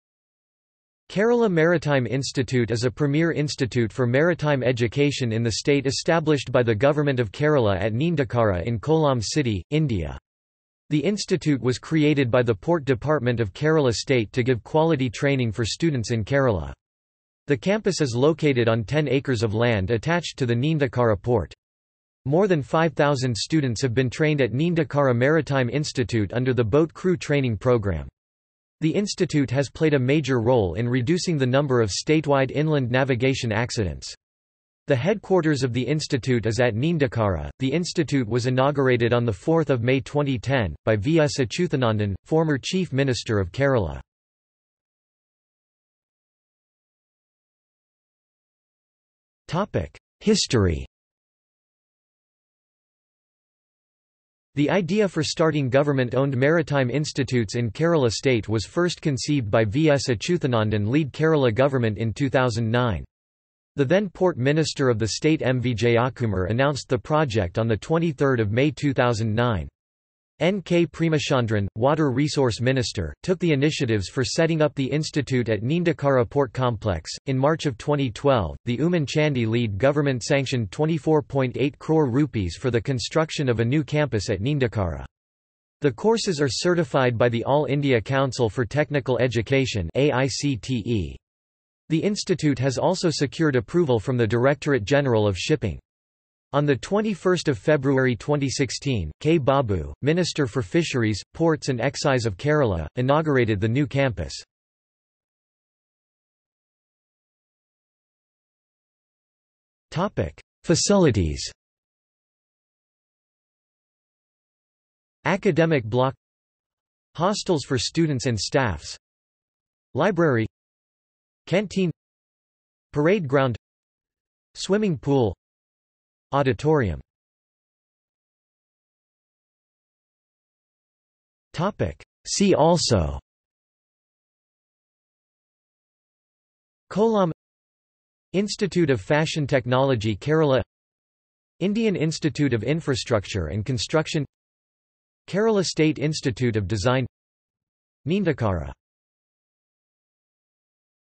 Kerala Maritime Institute | Wikipedia audio article
This is an audio version of the Wikipedia Article:
Kerala Maritime Institute

00:00:45 1 History
00:01:46 2 Facilities
00:02:07 3 See also

- Socrates

SUMMARY
Kerala Maritime Institute is a premier institute for Maritime education in the state established by the Government of Kerala at Neendakara in Kollam city, India. The institute was created by the Port Department of Kerala state to give quality training for students in Kerala. The campus is located on 10 acres of land attached to the Neendakara Port. More than 5,000 students have been trained at Neendakara maritime institute under the Boat Crew training programme. The institute has played a major role in reducing the number of state-wide inland navigation accidents. The headquarters of the institute is at Neendakara.The institute was inaugurated on 4 May 2010, by V. S. Achuthanandan, former Chief Minister of Kerala.